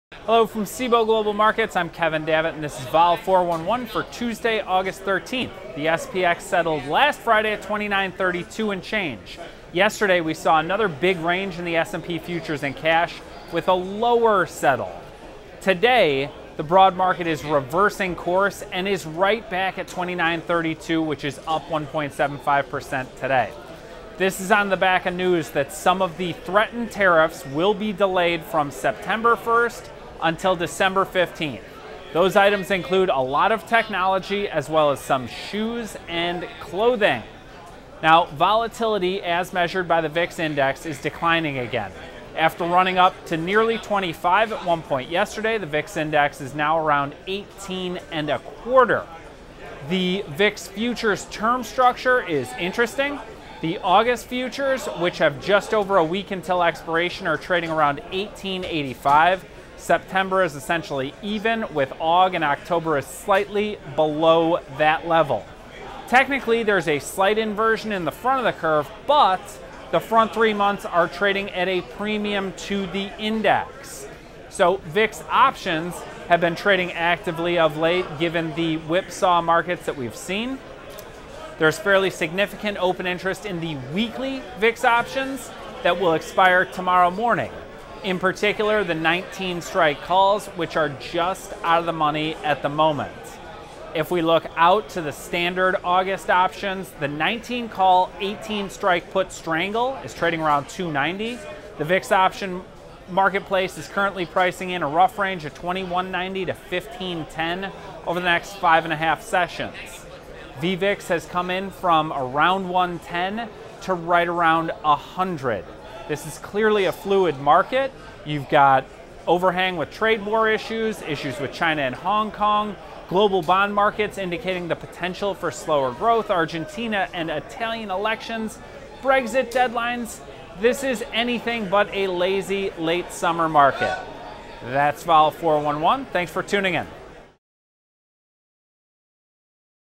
Market Reversal
Market reversal levels with last week's performance. Volatility anticipated ahead of awaited geo-political outcomes. Host Kevin Davitt